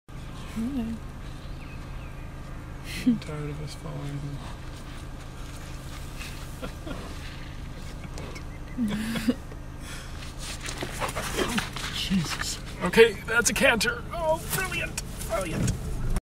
White rhino locomotion study
GPS unit to measure speed, attached to surcingle around belly. Unpublished research; see whatsinjohnsfreezer.com for more info in this work. Conducted at Woburn Safari Park, UK.
Movie by John Hutchinson, 2011.